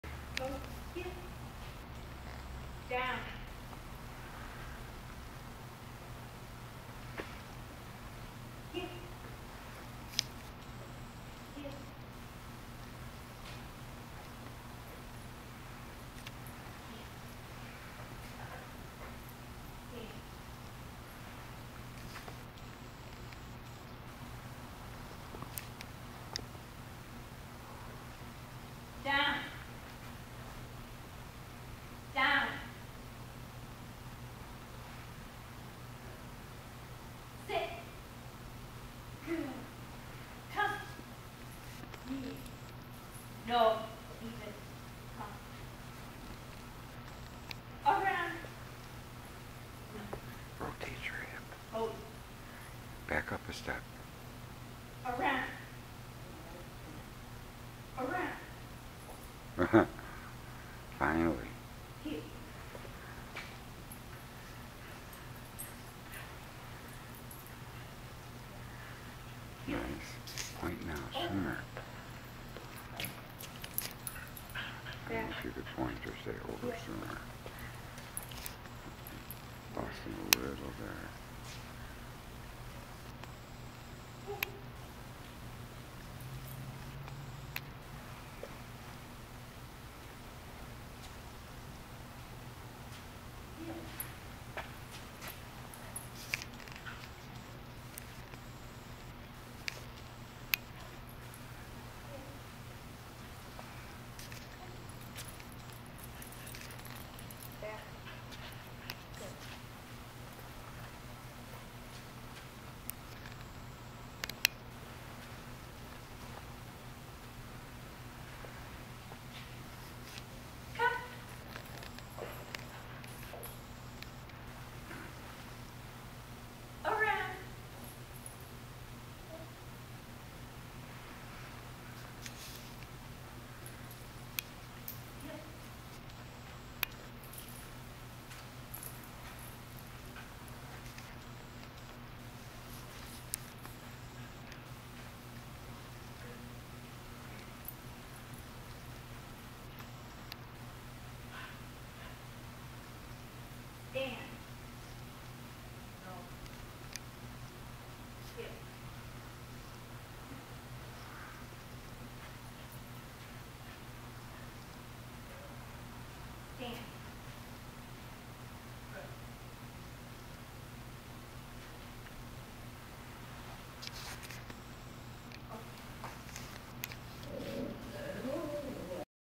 Allan Ross Dog Training
Jen and Bowie  practice a Cynosport Level 3 Rally Obedience course in the Canine College. 1